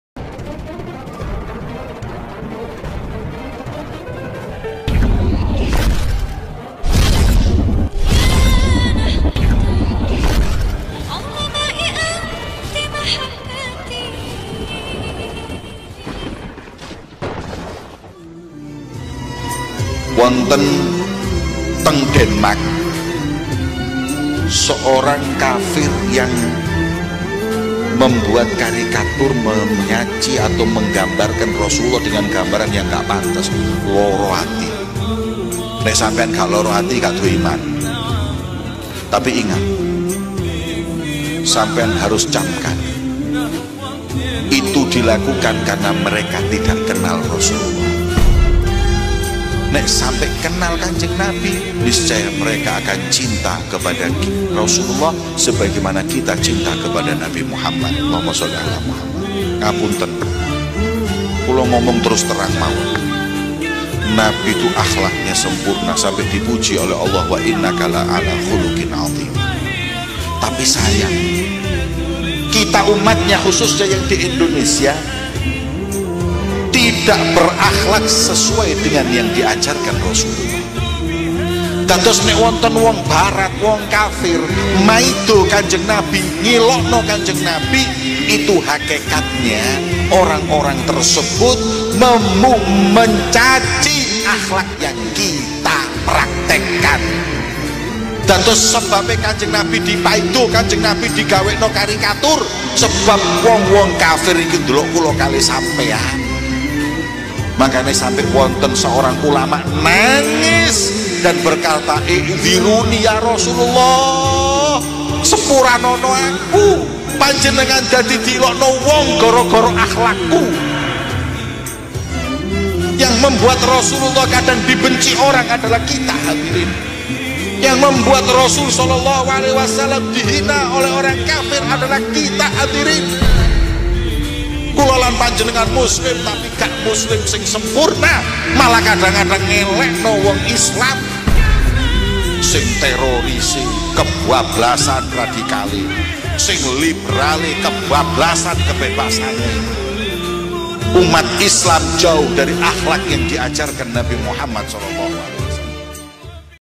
Prancis Hina Nabi Muhammad saw? Ini Tanggapan Habib Jamal Bin Thoha Baagil.
Kenapa selalu ada silih berganti bermunculan penghina Nabi kita?